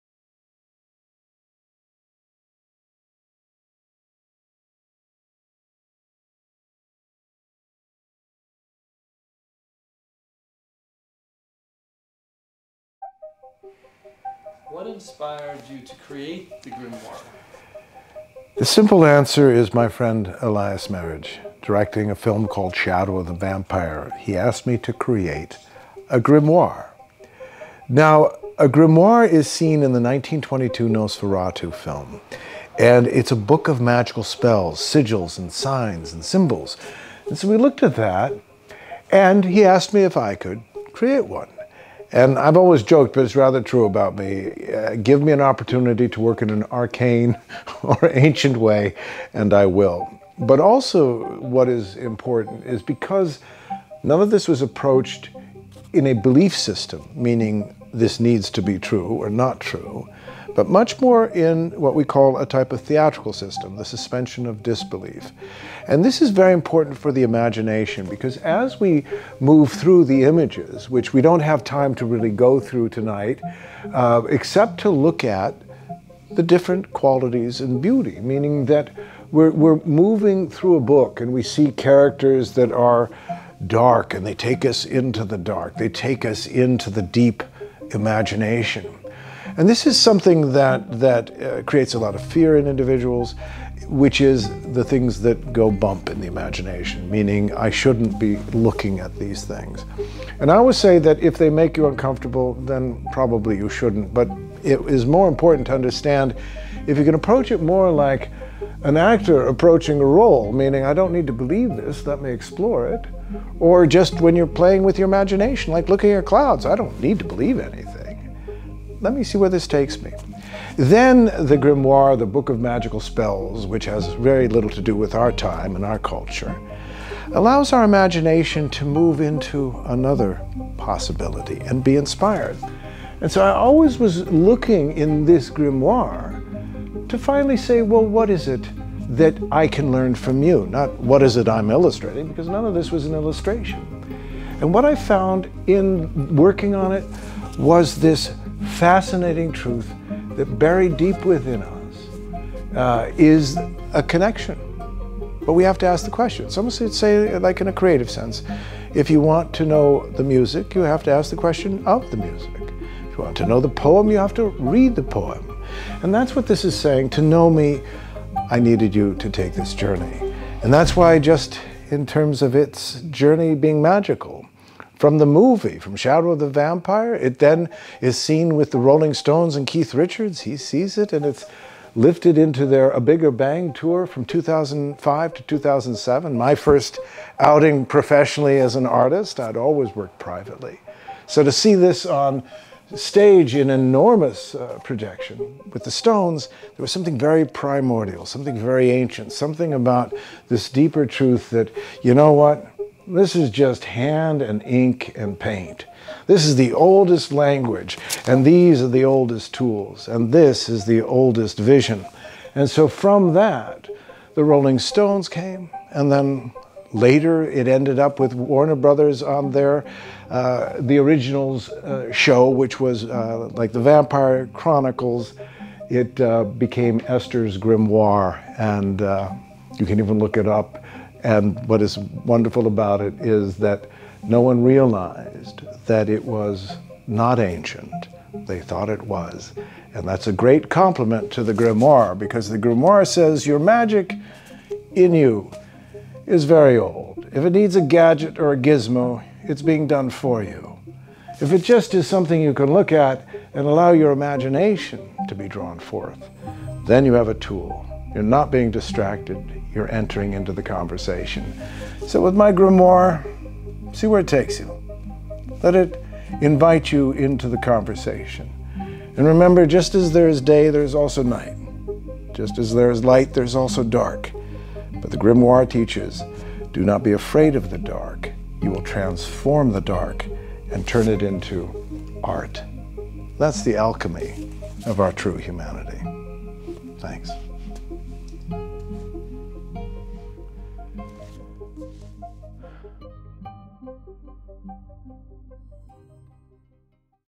THE GRIMOIRE AND THE ROLLING STONES: An Unexpected and Most Magical Journey with Leigh J McCloskey.
From touring on stage with Keith Richards and the Rolling Stones on their “A Bigger Bang World Tour” (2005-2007), the journey of Leigh’s hand painted GRIMOIRE (Book of Magical Spells) has been remarkable and it all began by doing a good turn for a dear friend. The 18 page folio book was originally created for Willem Defoe’s vampire character in the film, ”Shadow of the Vampire” directed by a dear friend, E.Elias Merhige who suggested it. The Grimoire even ended up as “Esther’s Grimoire” on, WB’s,”The Originals.” This short immersive look is a great way to explore the richness of the art and insights into the story and meaning and the unexpected magic of the Grimoire.